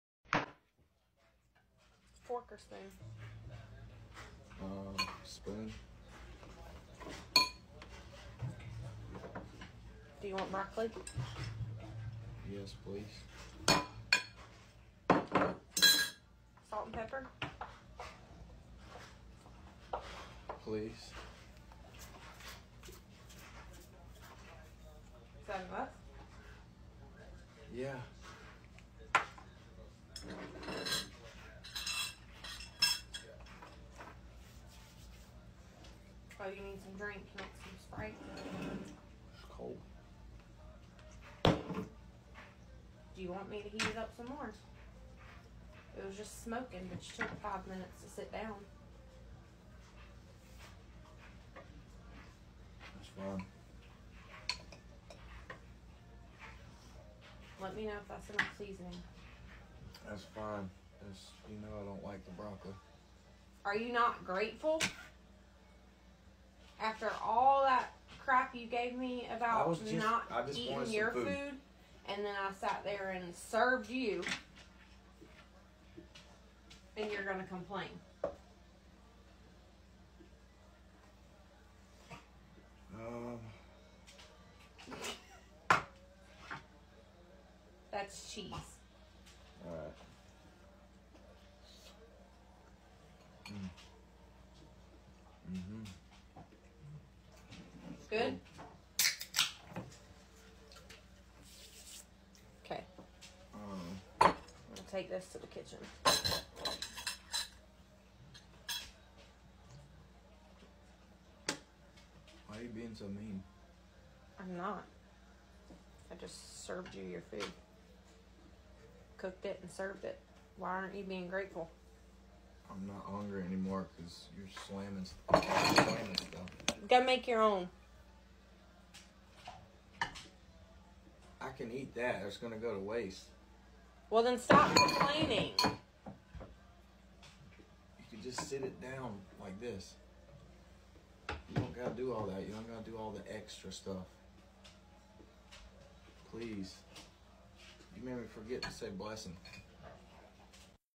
med Crazy Hillbilly Gaming Endnu en TikTok “prank” mod mænd
med Crazy Hillbilly Gaming Endnu en TikTok “prank” mod mænd. Hvorfor er det sjovt? Det havde været kaldt psykosk v0ld hvis en mand gjorde det mod hans partner